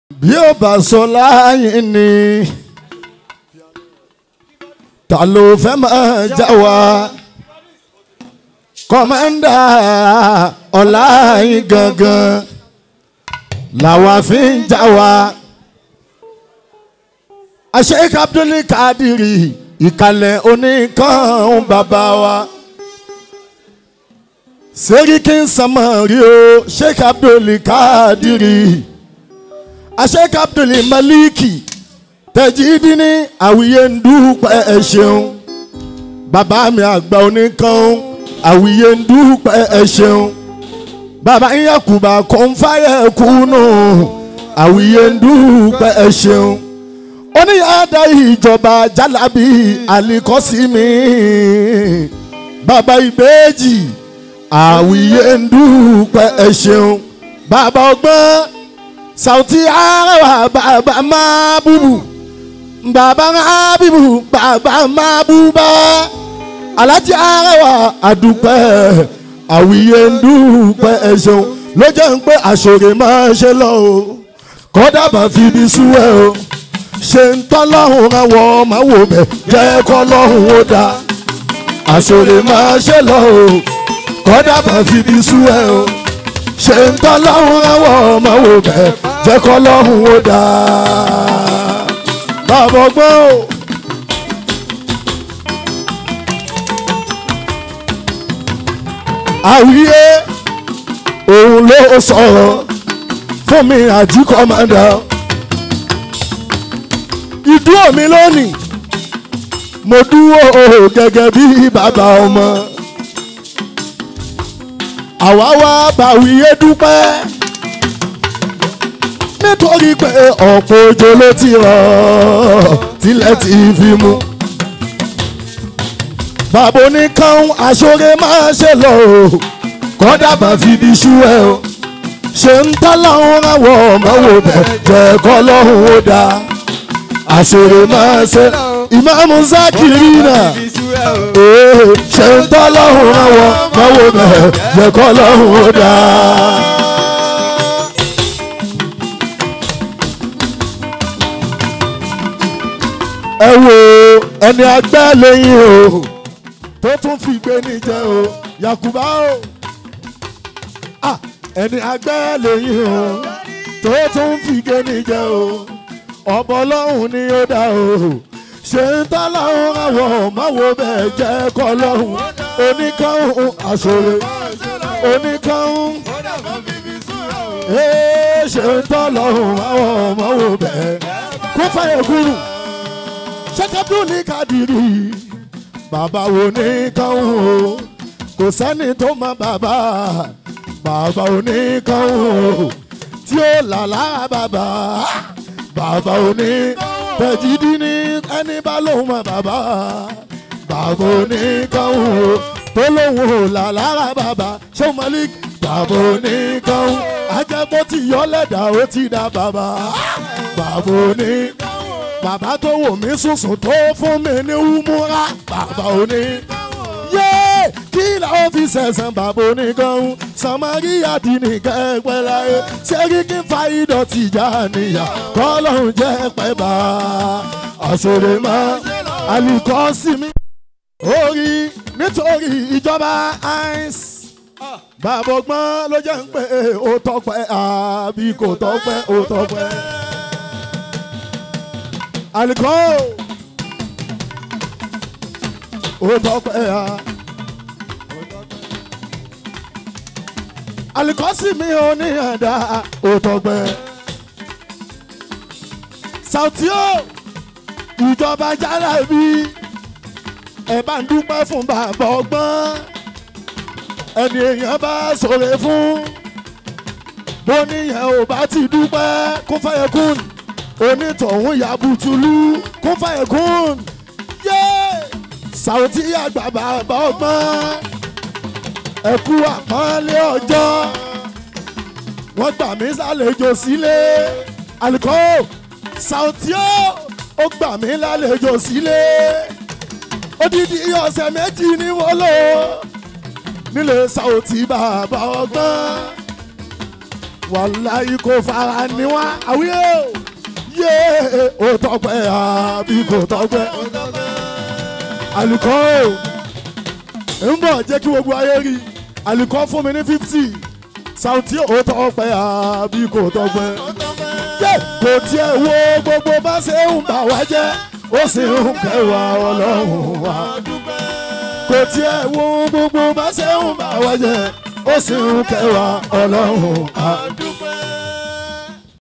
LIVE PERFORMANCE ALH. ABDUL RASAK YUSUF COMMANDER AT AWIYE AGBA DAY 2021
LIVE PERFORMANCE ALH. ABDUL RASAK YUSUF COMMANDER
at Awiye Agba 4 in 1 Program. Awiye Agba Day, Launching of New Album, Award Presentation and Inauguration of Awiye Agba Fans on the 12th of September 2021.